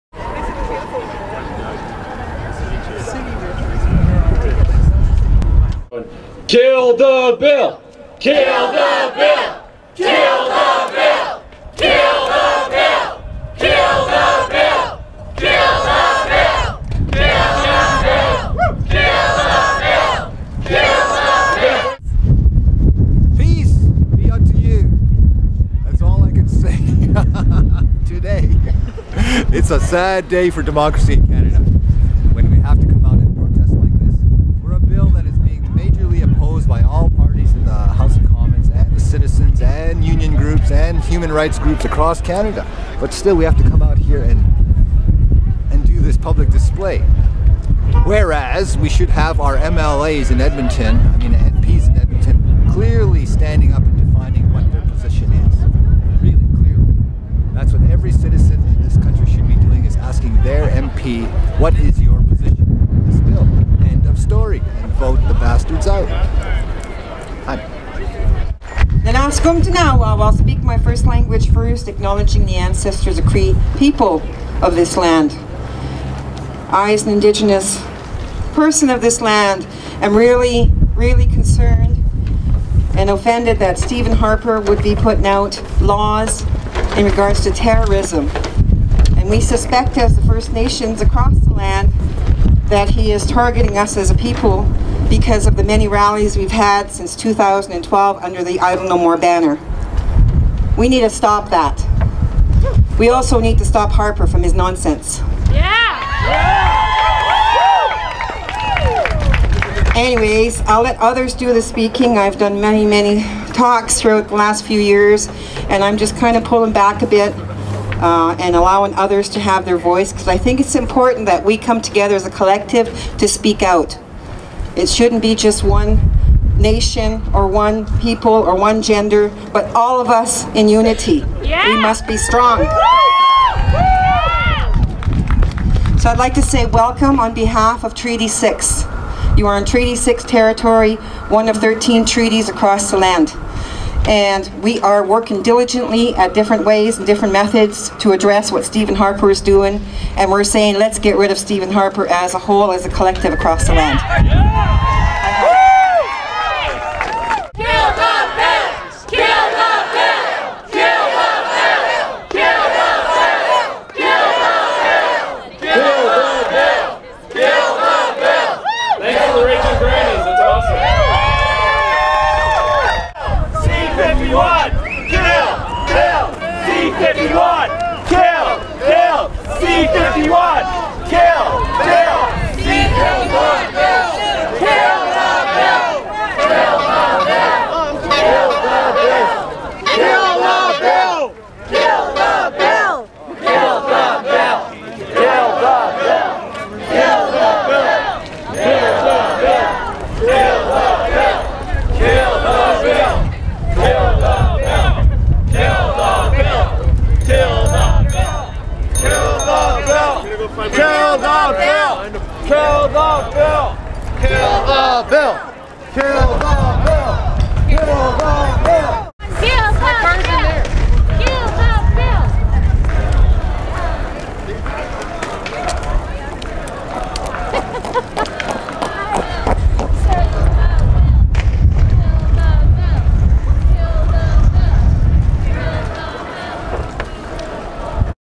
Stop Bill C-51 - Edmonton - National Day of Action
Chanting, marching, a short interview with an Aboriginal activist, and an opening prayer at the Stop Bill c-51 National Day of Action in Edmonton on April 18, 2015.